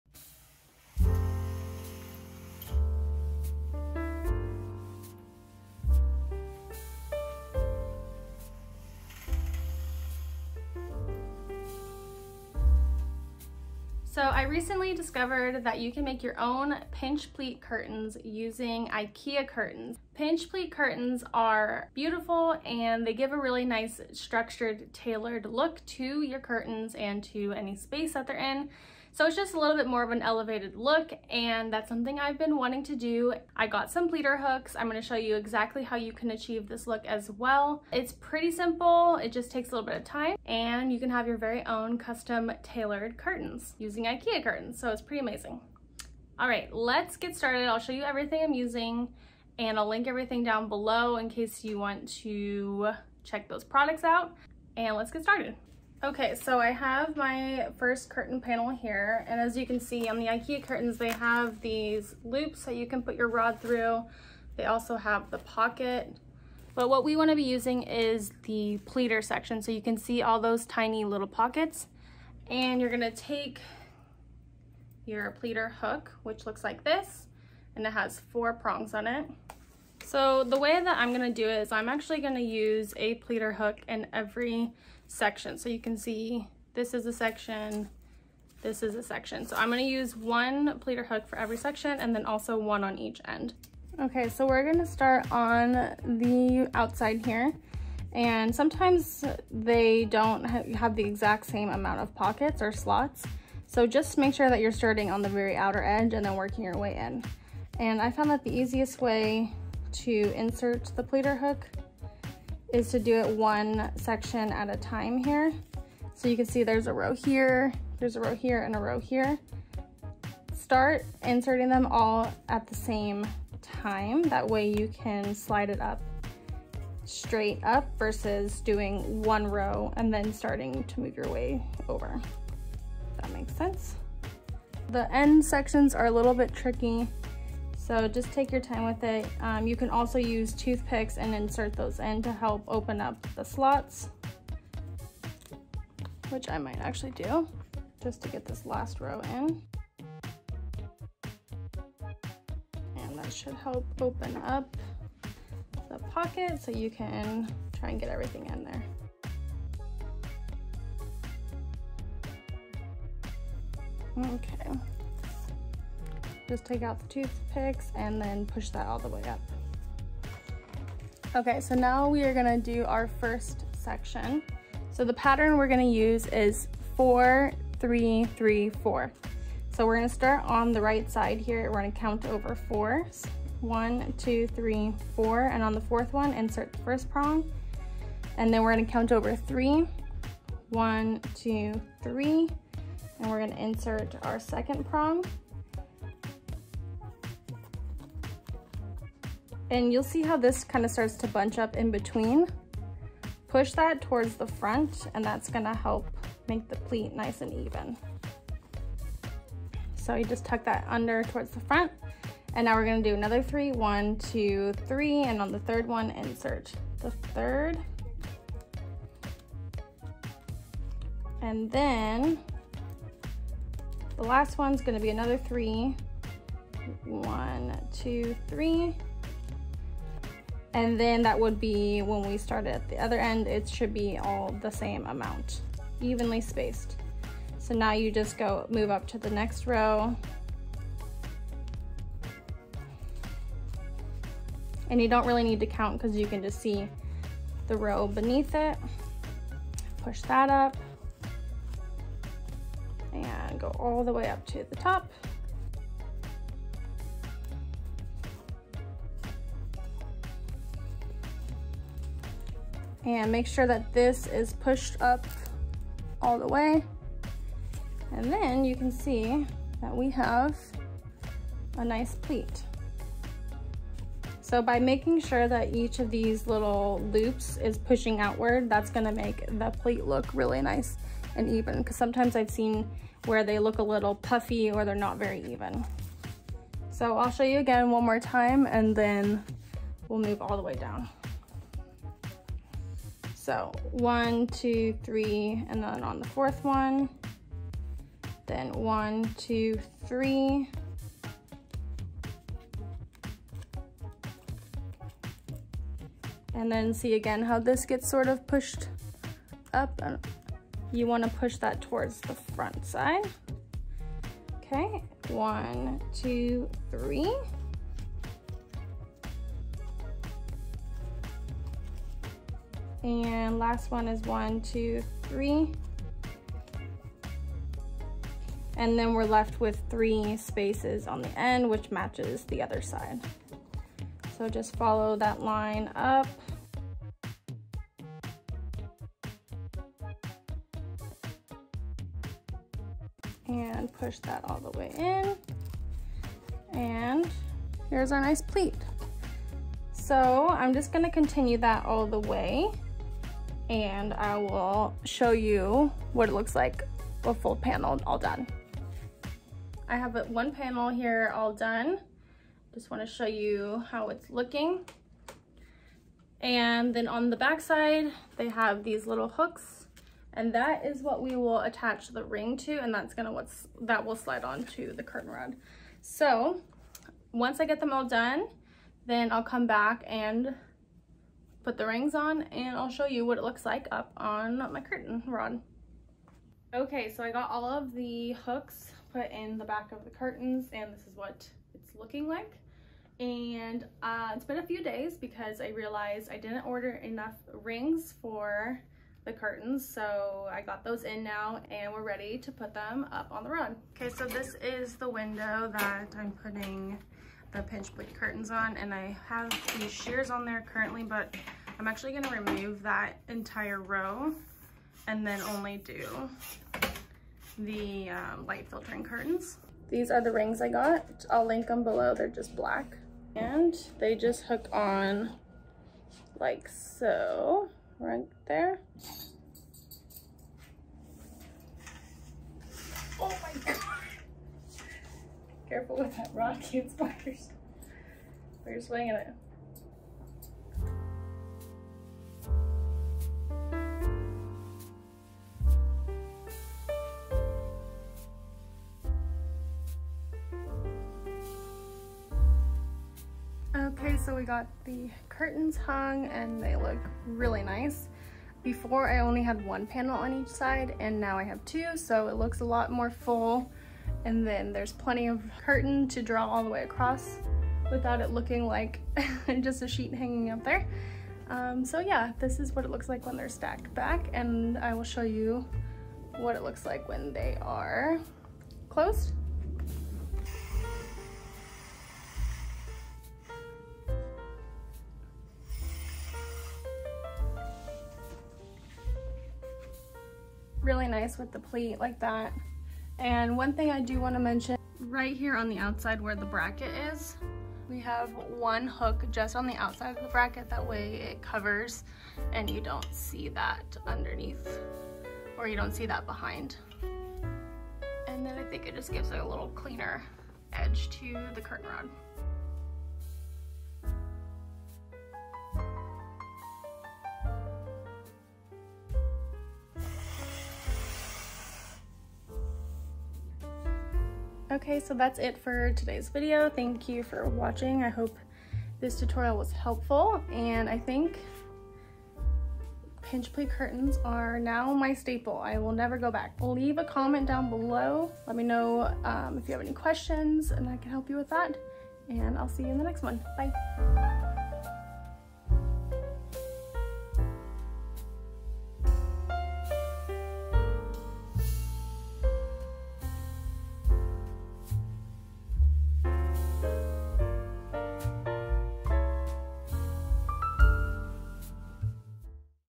DIY Pinch Pleat Curtains | IKEA HACK
In this video, I show how I used IKEA curtains to make pinch pleat curtains. This is a straightforward and affordable way to upgrade your curtain game and get on that designer level. I've had these IKEA curtains for years, and I wish I had known this hack sooner. But I couldn't be happier with the result!

NOTE: Each of my IKEA curtain panels needed 11 pleat hooks and 11 rings. My curtains are 57" x 98".

curtains - any IKEA curtain will work as long as it has the pleat tape on the back.
curtain rings
pleat hooks